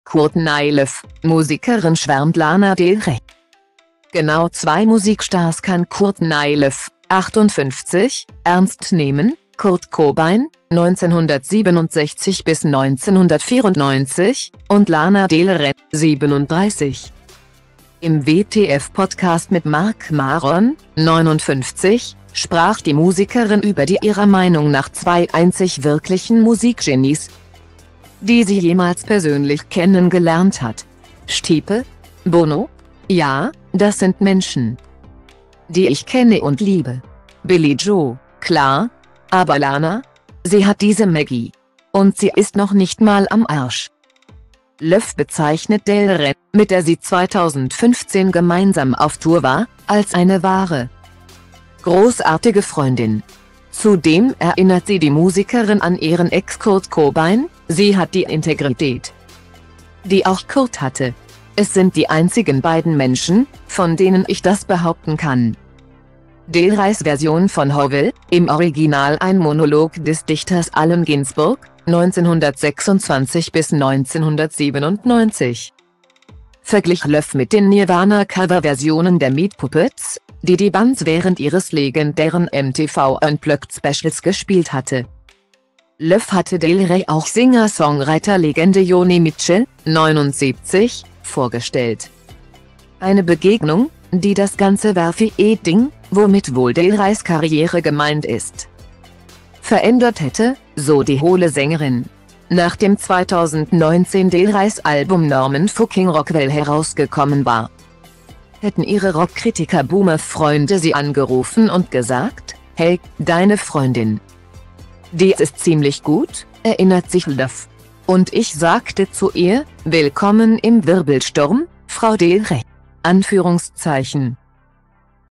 Courtney Love: Musikerin schwärmt Lana Del Rey
Genau zwei Musik-Stars kann Courtney Love (58) ernst nehmen: Kurt Cobain (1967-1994) und Lana Del Rey (37).Im "WTF Podcast" mit Marc Maron (59) sprach die Musikerin über die ihrer Meinung nach zwei einzig wirklichen Musik-Genies,die sie jemals persönlich kennengelernt hat. "Stipe? Bono? Ja, das sind Menschen,